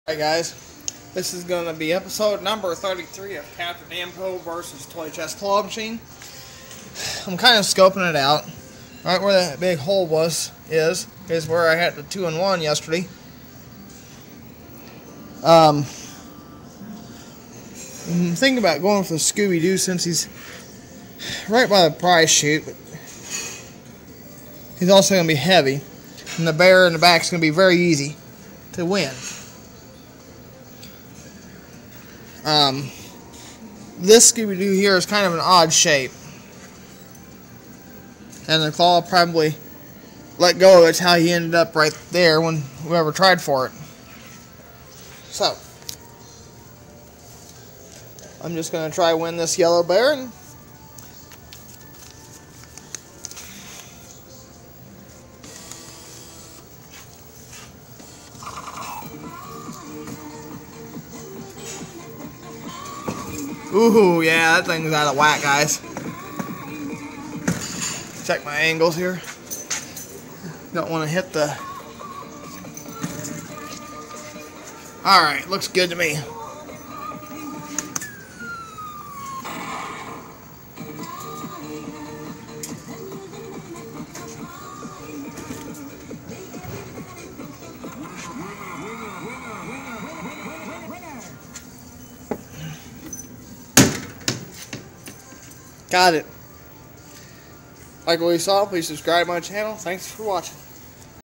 CAPTAIN NAMCO VS TOY CHEST CLAW MACHINE EPISODE 33 CHECKIN THOSE ANGLES
THIS WAS FILMED ABOUT A WEEK AGO AND HERE IS MY 33RD EPISODE OF CAPTAIN NAMCO VS TOY CHEST I HOPE U ENJOY